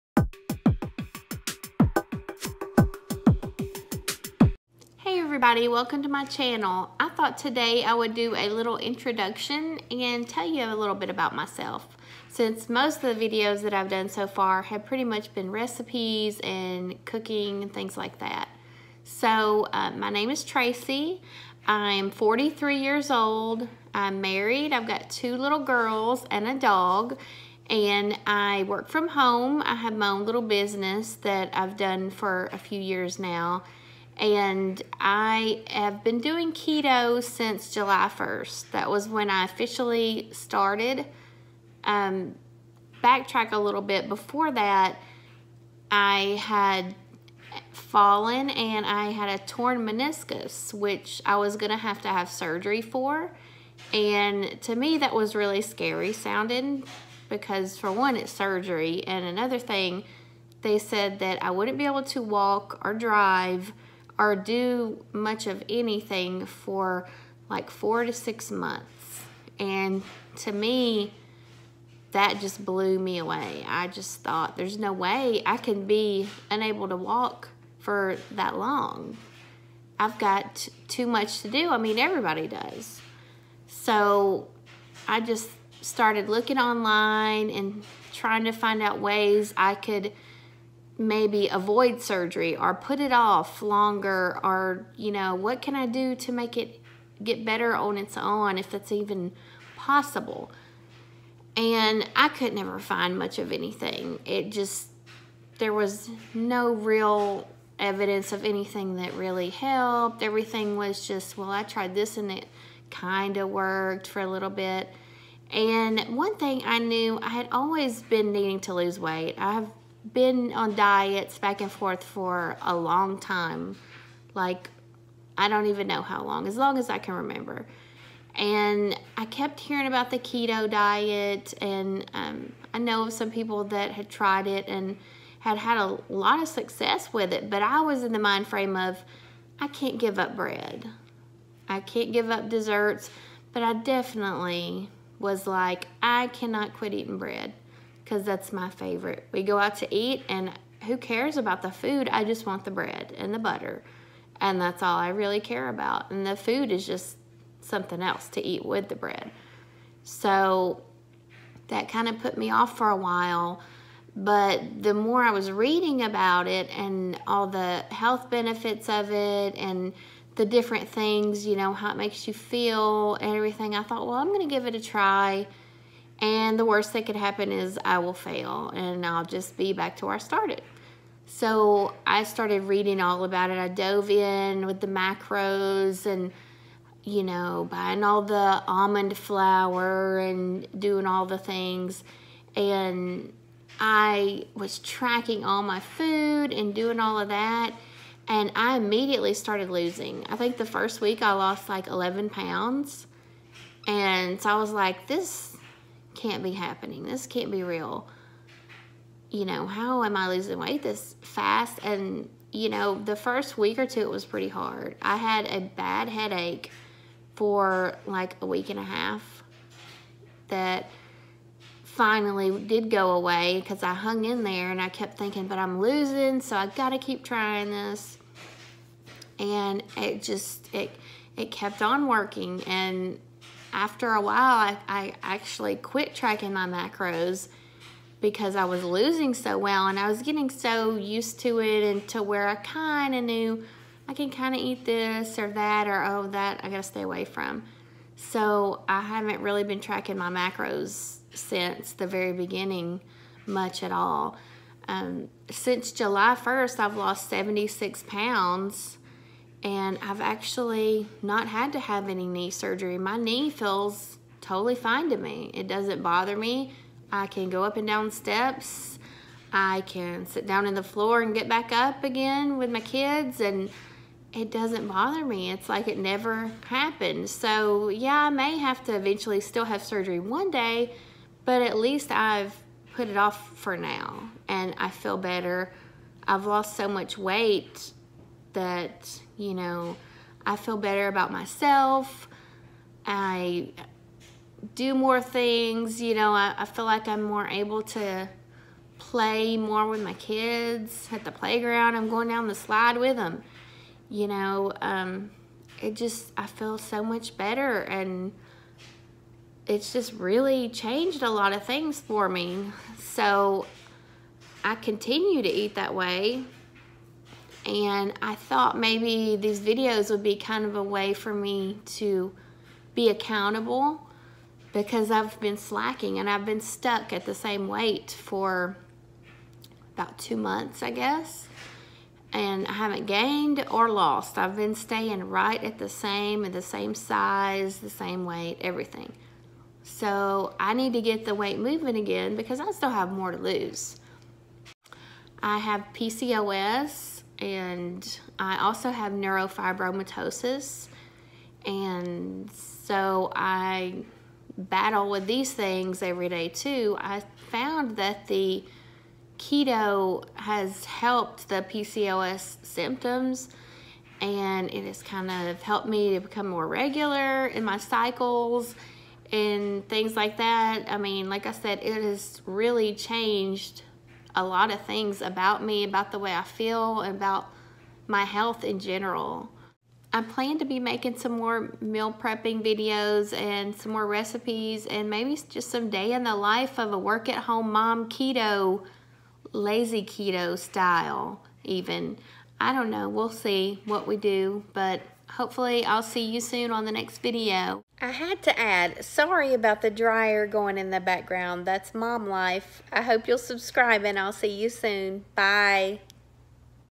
Just a little about me keto life wahm only one keto at my house
Yes, I’ m awkward! This is a little intro about me and my channel. I hope you stick around for more of my videos